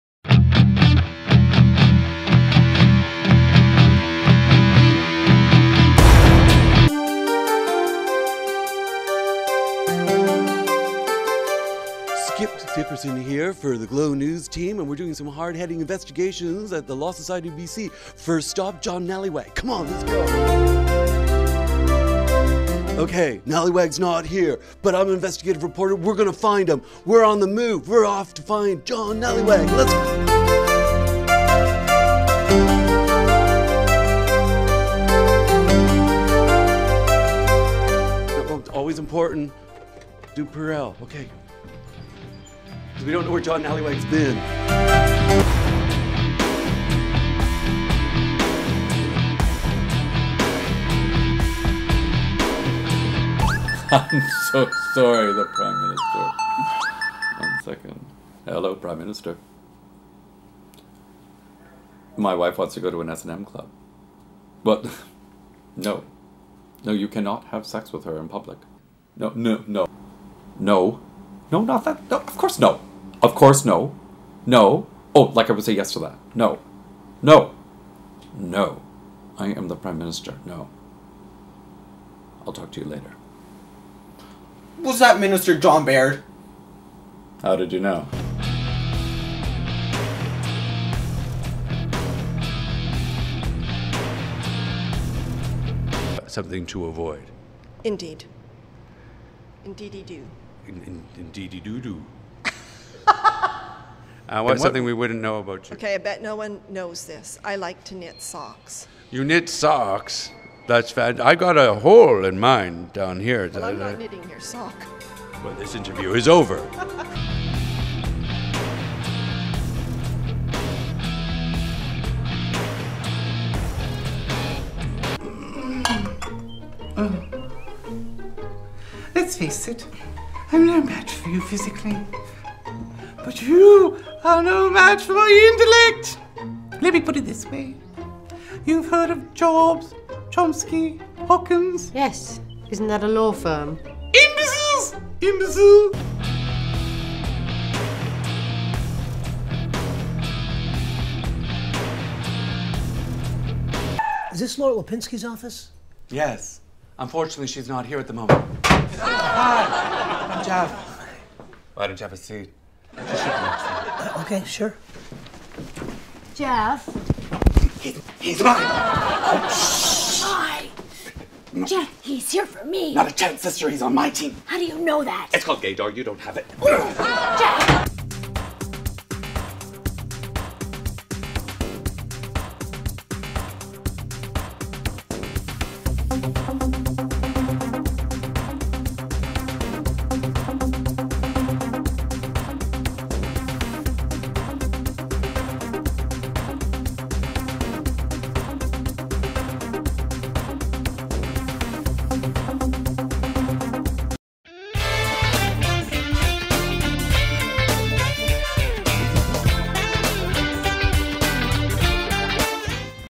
David C. Jones Comedy Characters Part One
Here are some filmed characters I have done.

I will so be uploading a series of comedy monologues.